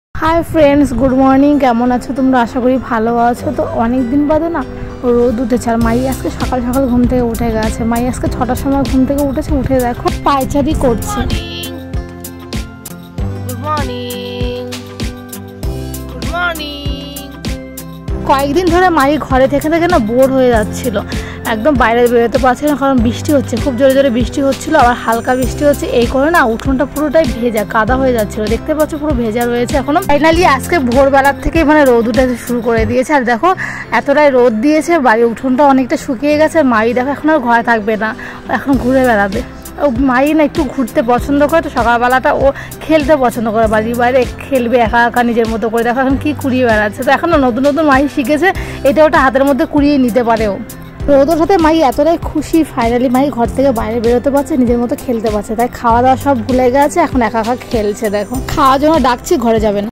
অনেকদিন বাদে রোদ উঠলো আর মাহি কতটা খুশি দেখো! খাওয়া-দাওয়া ভুলে গিয়ে মাহি খেলা করছে রোদ উঠেছে তাই
রোদ ওঠার আনন্দে মাহি খাওয়া ভুলে গেছে